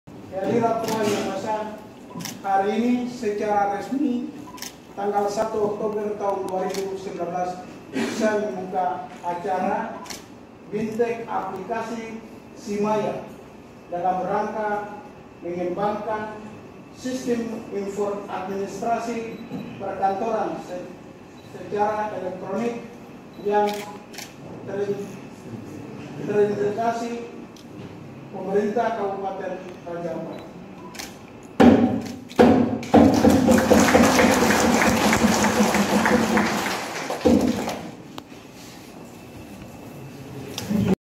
DISKOMINFO RAJA AMPAT GELAR BIMTEK ALPLIKASI SIMAYA
Raja Ampat, Diskominfo_ Pemerintah Kabupaten Raja Ampat melalui Dinas Komunikasih Informatika persandian dan Statistik gelar Bimbingan Teknik dalam Rangka mengembangan sistem administrasi perkantoran secara elektronik yang terintegrasi di Pemerintah Kabupaten Raja Ampat di Gedung Aula Kantor BAPEDA Raja Ampat, Selasa, (1/10/2019). Asisten III Setda Kabupaten Raja Ampat Yulianus Mambraku, SH, M.Si,  mengatakan  Aplikasih simaya ini adalah administrasi perkantoran berbasis web , maka proses manajemen persuratan yang biasanya dilakukan dengan manual dapat dilakukan secara otomatis sehingga memudahkan dalam proses Adminstrasi, pencarian dan pengarsipan , dimana sebuah langka yang maju bagi daerah ini untuk memanfaatkan perkembangan teknologi dalam rangka menciptakan efektivitas, akuntabilitas dan transparan. Iapun  berharap peserta agar mengikuti kegiatan bimtek ini dengan serius.

" saya berharap bagi peserta agar mengikuti kegiatan ini dengan serius sehingga ke depan bisa bekerja sebagai admin yang mampu mengola tugas dan penyelenggaraan roda di OPD masing masing." Katanya.